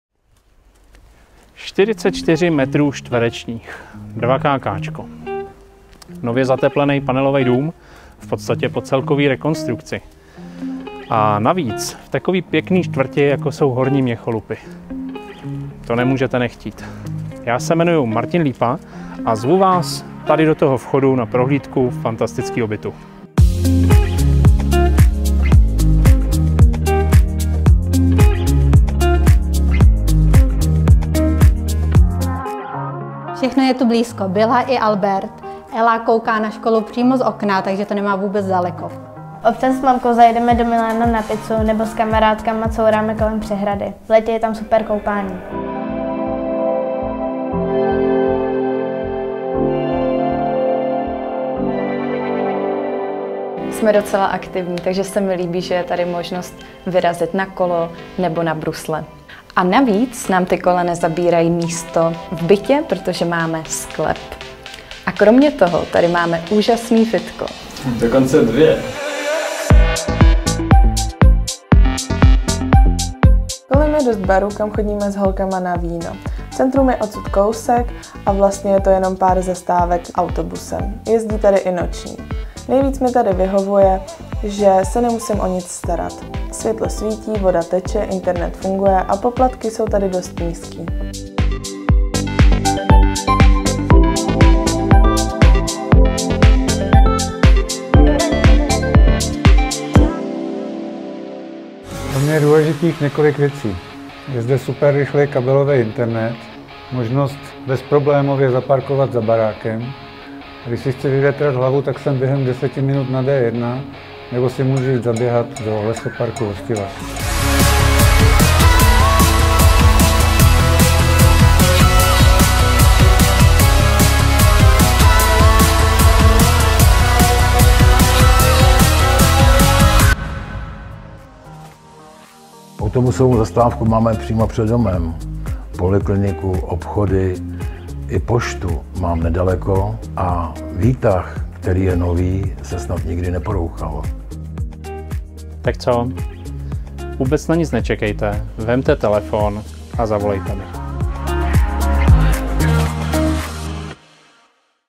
MARTIN LÍPA: Pěkný čistý byt 2kk v Měcholupech
Od bytu 2kk o klasické výměře 44m2 a snad nejběžnějším půdorysu "ložnice rovně, obývák vlevo, kuchyň za rohem" v panelovém domě většina z nás očekává několik základních věcí:

- aby byl byt čistý a světlý. To tento je. V obýváku a v ložnici jsou nové koberce, v předsíni lino, byt je čistě vymalovaný, okna jsou plastová a velká.

- aby na něj pokud možno nebylo potřeba sáhnout. Nemusíte - kupte si ledničku, trochu nábytku a přijďte bydlet.

- aby bylo hotové jádro. Zde je koupelna společná s toaletou, vše v Ytongu. Voda i odpady v plastu, elektřina v mědi a obklady jsou taky v pohodě.

- důležité je i mít kam dát věci. To zde zajistí velká vestavěná skříň v předsíni a pokud by se něco nevešlo, tak to dáte do sklepa. Ten je plechový, bytelný, žádné plaňky nebo pletivo.

- dům by měl mít zdravé finance a být zateplený a celkově v pořádku. To tento je, navíc prošly rekonstrukcí i interiéry a výtah, hotové je vše, na co si vzpomenete.

- poplatky za služby jsou kolem 3000 včetně fondu oprav, přesná výše záleží na tom, kolik vás v bytě bude. Pokud jen jedna osoba, bude to ještě méně.

Bydlení v Ravennské je fajn hlavně kvůli okolí. Tři supermarkety jsou co by kamenem dohodil a zbytek došel, v okolí je spousta různých restaurací i barů a až se do nich bude moci zase chodit, tak je určitě neváhejte navštívit.

Autobusy včetně nočního spoje stojí hned u začátku domu a na metro Háje nebo Skalku se dostanete za 13 minut. Pokud radši jezdíte autem, bude vám radost dělat parkoviště přímo v Ravennské, případně můžete zajet za roh a zaparkovat i ve slepé ulici, která obtáčí dům. Problémy s parkováním vás tady prostě nečekají.

Jestli máte rádi přírodu, tak vězte, že Měcholupy jsou celkem zelené sídliště, ale pokud by vám to bylo málo, za pár minut jste v Hostivařském lesoparku nebo u přehrady. A tam je i super koupání.

Byt je v osobním vlastnictví bez právních vad, daň z nabytí už byla konečně zrušena, takže pořízení není spojené s žádnými dalšími poplatky.

Pokud chcete hypotéku, rádi vám tu nejvýhodnější pomůžeme zařídit.

Těším se na vás na prohlídce.